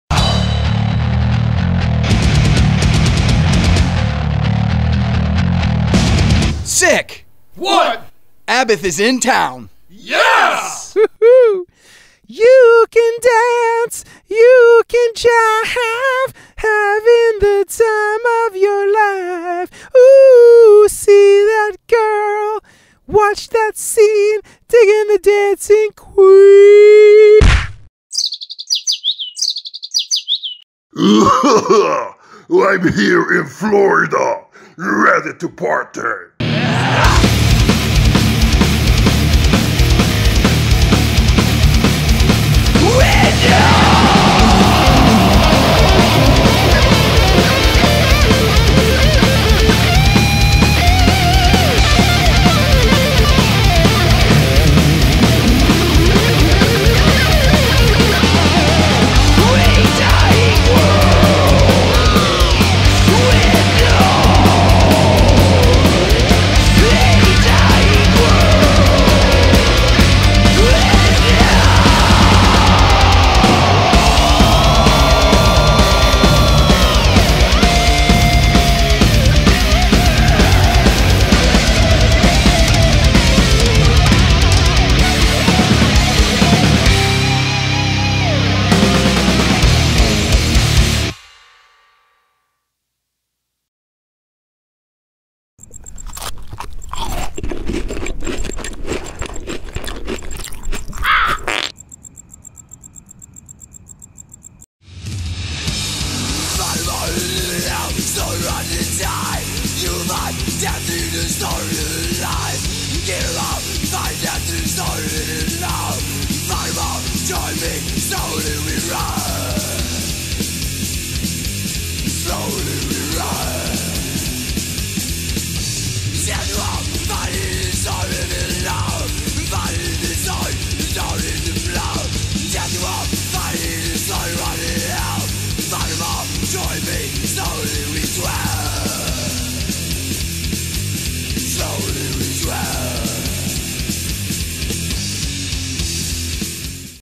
OBITUARY - 30 Years Rotting Slow w / ABBATH
Catch OBITUARY and celebrate 30 Years of Rotting Slow with Abbath, Midnight & Devil Master!